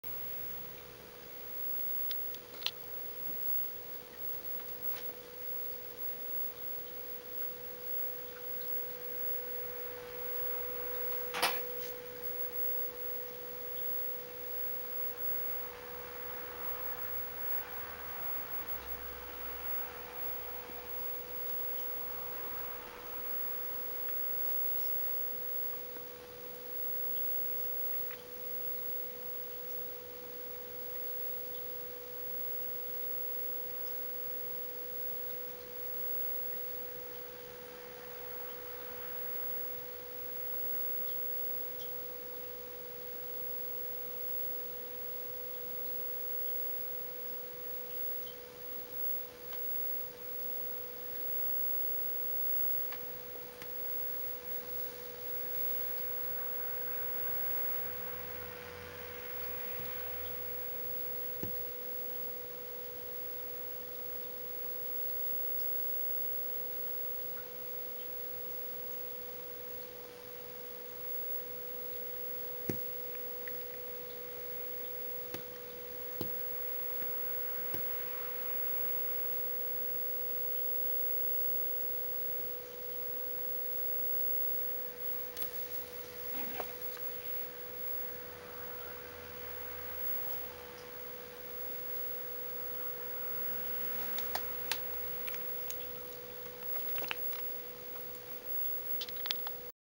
爆睡するニノ
でかくて頭がはみ出しておるwwwそして起きない！